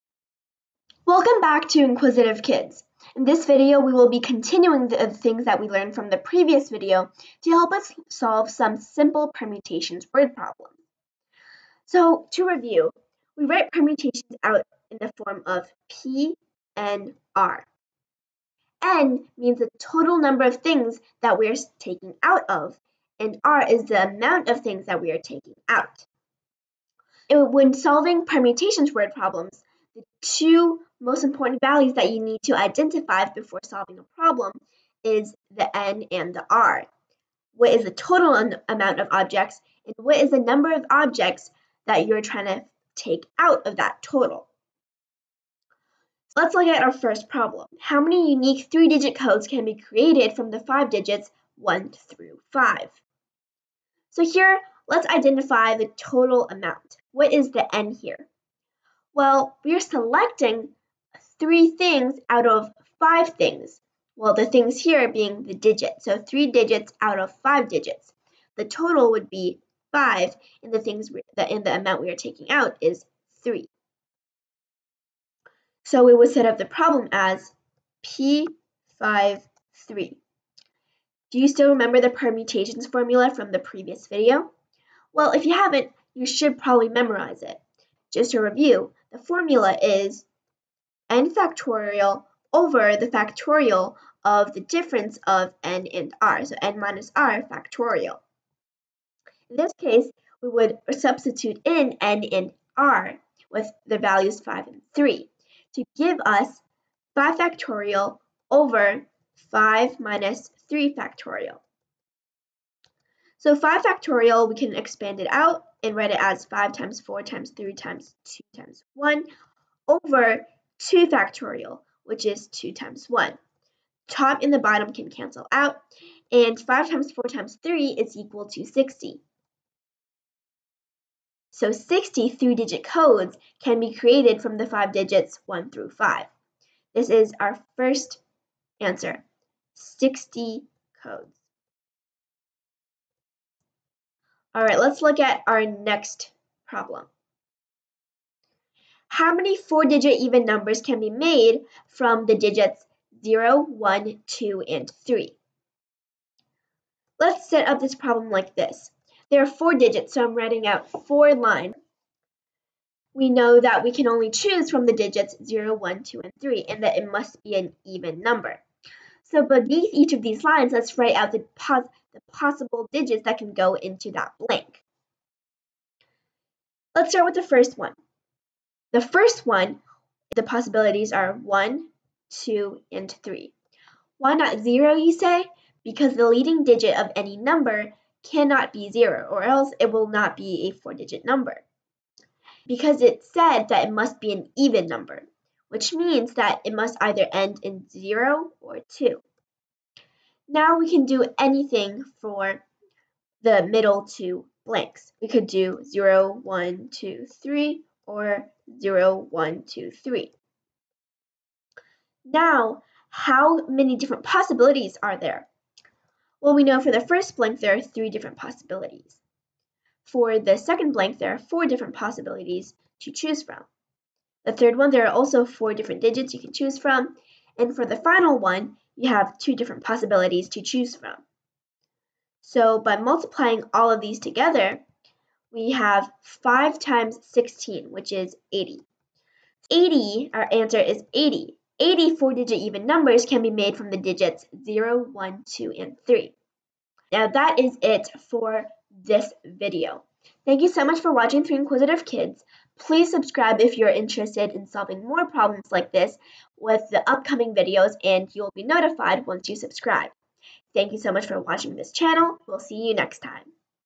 AMC8 Course | Permutations | Problem 1
AMC8 is a competitive math competition for students in the eighth grader and below. Anyone under this age condition can take the AMC8, and just like the MOEMS competitions. In this course you will learn all of the basic topics and skills necessary to let you succeed in middle school competitive math.

In this video, we will be covering the basic strategy to solving permutations word problems.